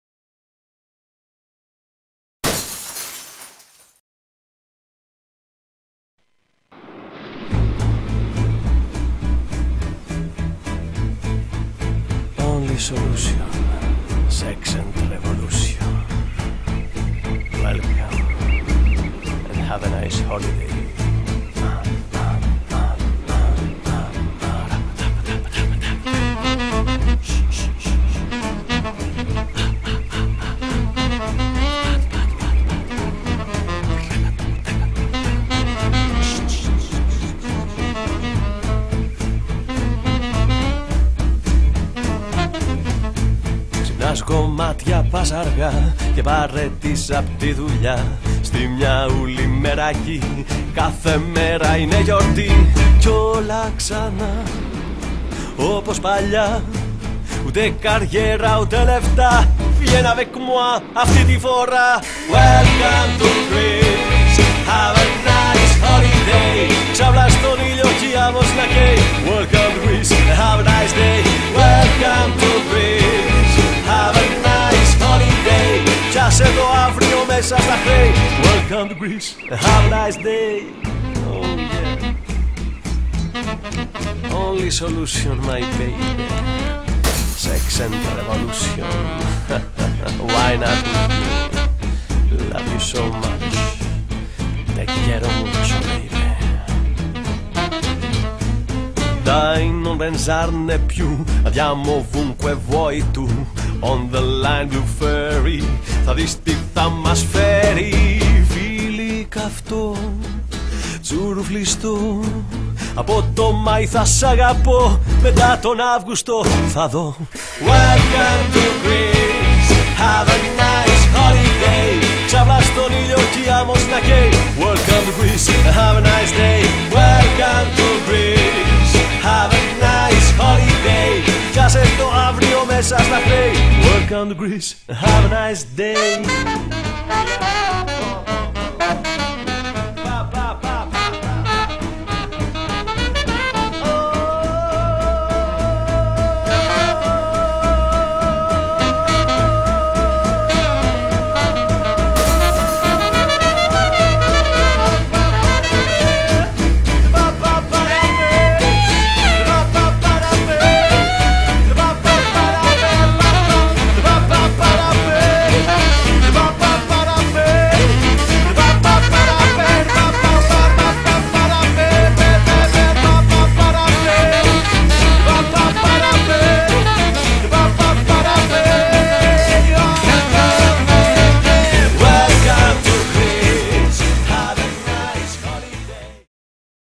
That's Greece... rising!
Tourism in the Greek Islands is rising and the Greek attitude is changing. More and more Greeks support ecology, protest peacefully and embrace Modern Greek culture!